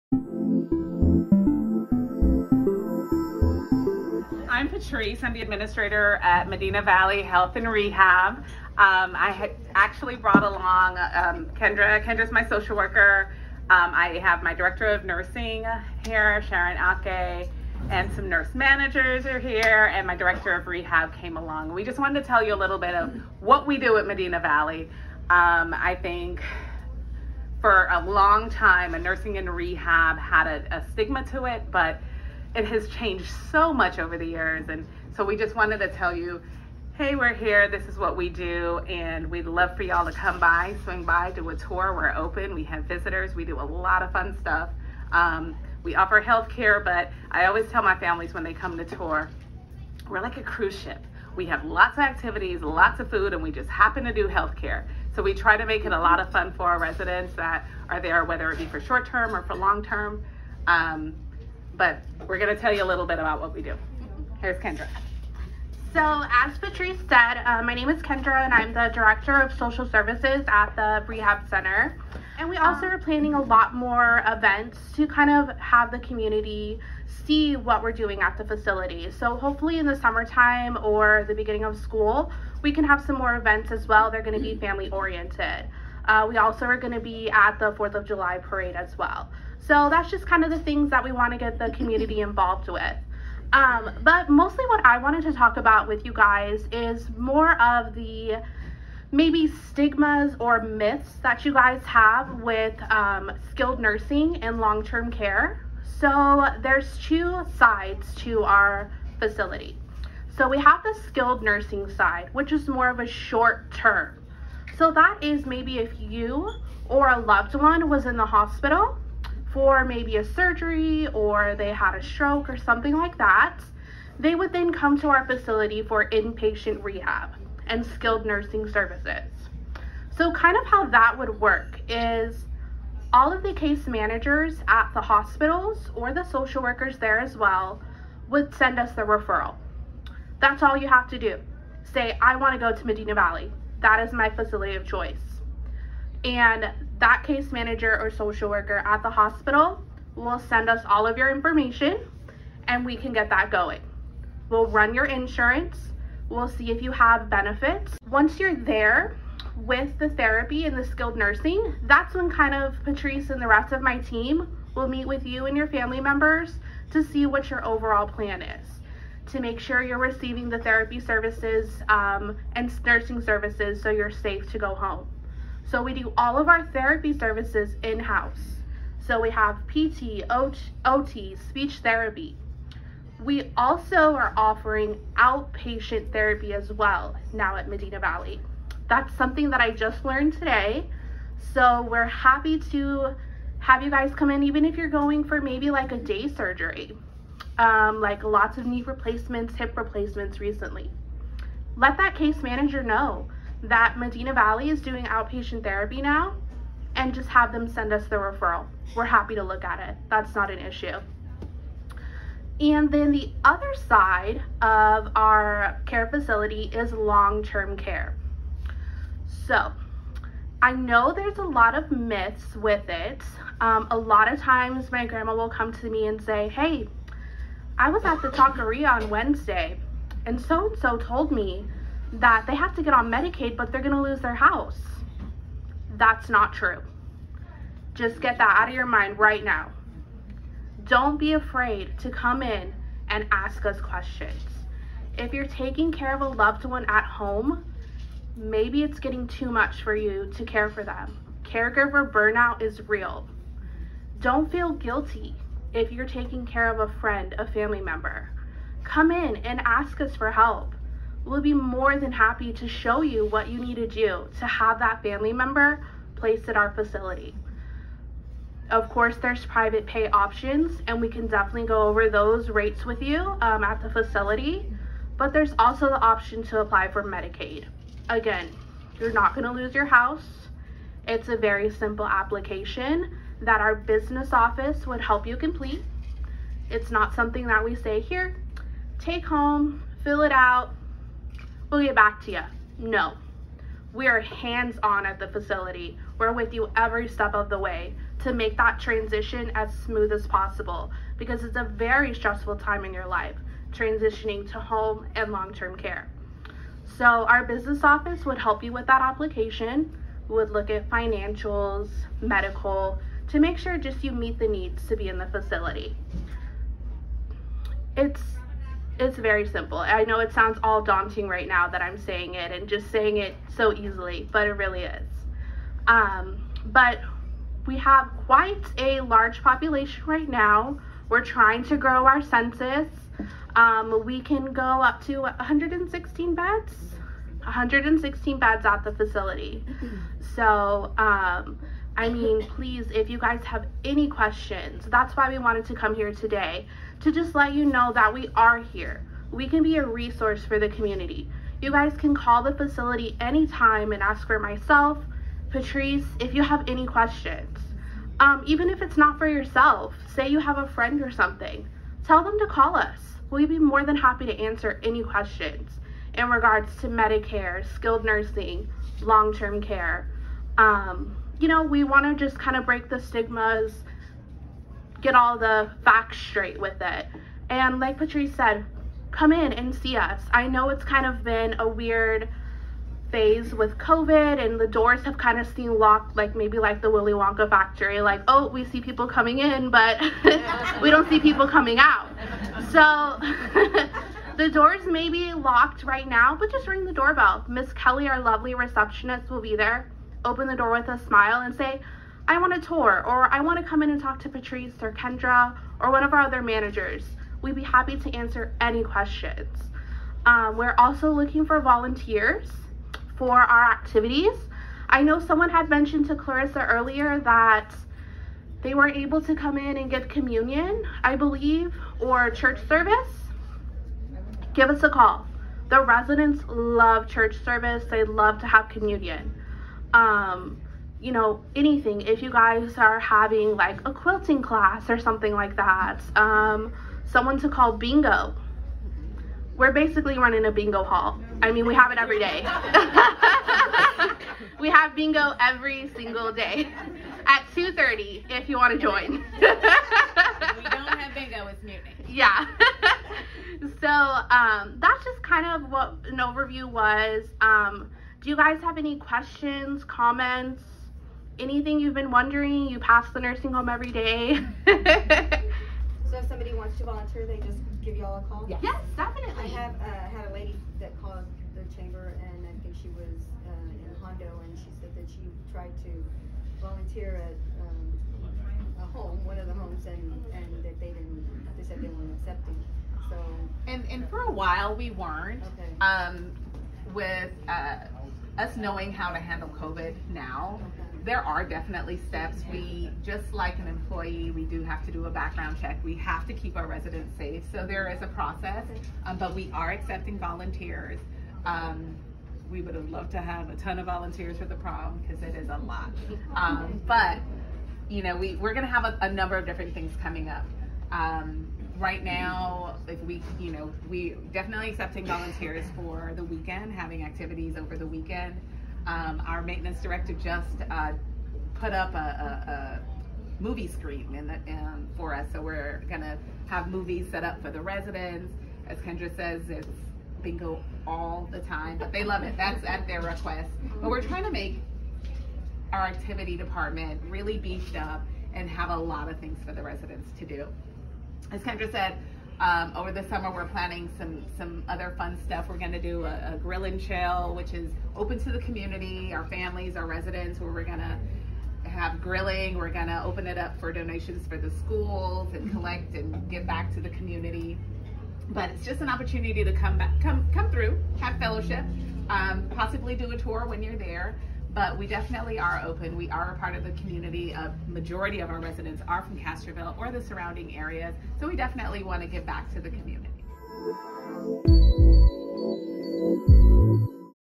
Medina Valley Health & Rehab
If you missed our member luncheon last week, Medina Valley Health & Rehab addressed some of the myths regarding skilled nursing and long-term care. They also filled us in on all the fun activities happening at their facility and in the community.